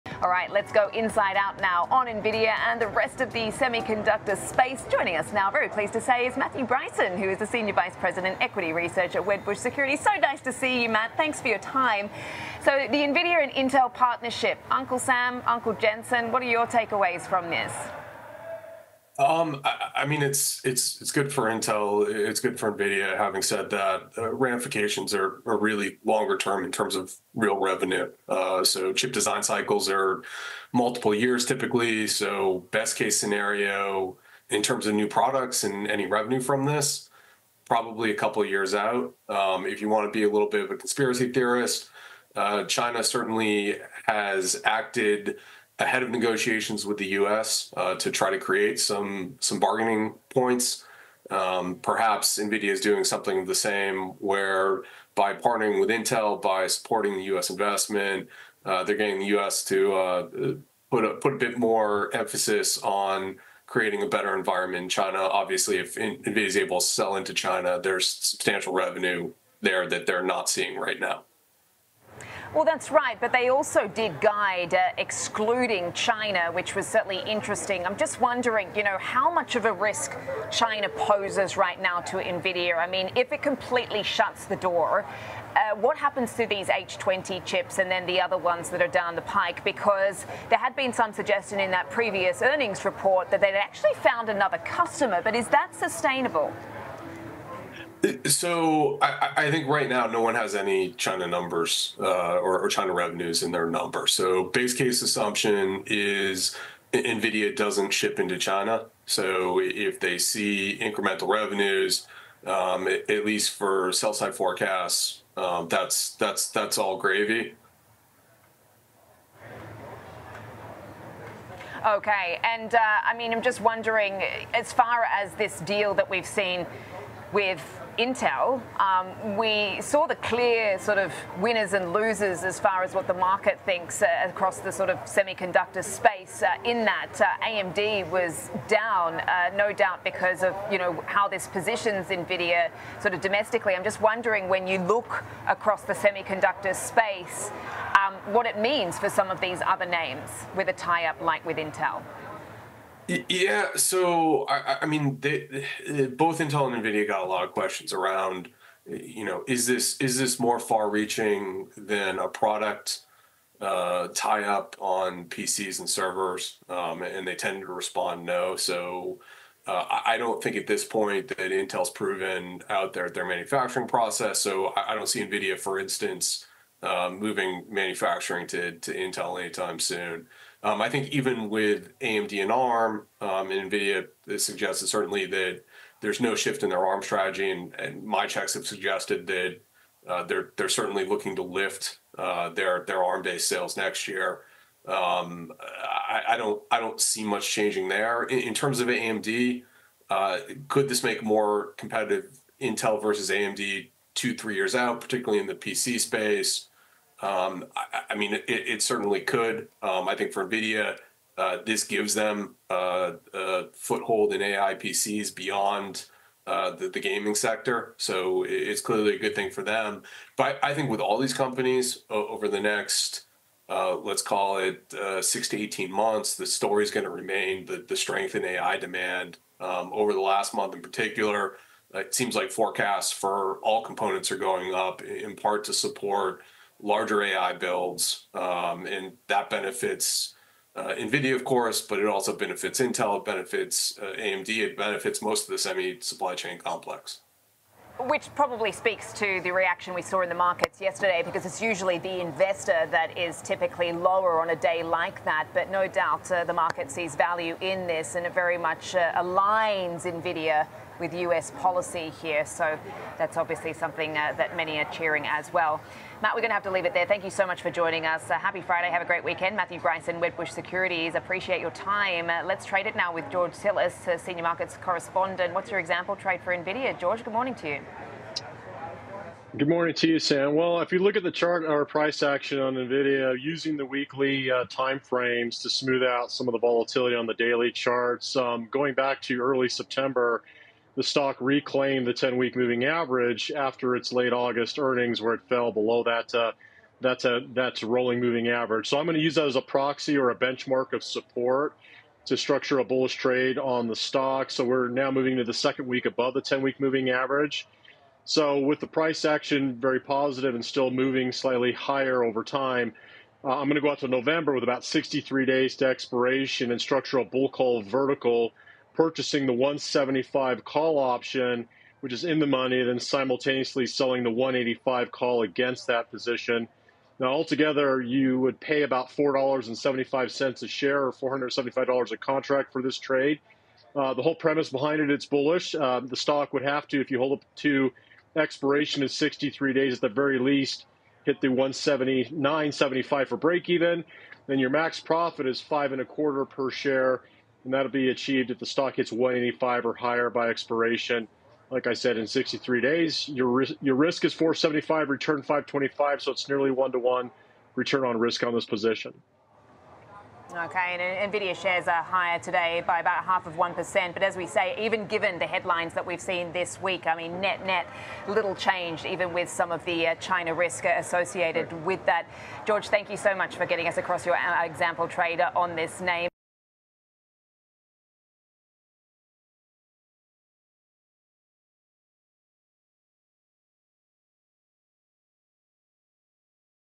NVDA's Week That Was: China Issues, INTC Investment
Matthew Bryson says China may have intentionally acted ahead of negotiations with U.S. officials when its regulators began calling for stops to Nvidia (NVDA) chips. He joins Sam Vadas to recap a busy week for Jensen Huang's A.I. chipmaker as the company faces potential headwinds from the Chinese markets. Matthew also weighs in on the company's Intel (INTC) investment and what that means for domestic semiconductor production. Later, George Tsilis maps out an example trade for Nvidia.

Schwab Network
Options involve risks and are not suitable for all investors. Before trading, read the Options Disclosure Document.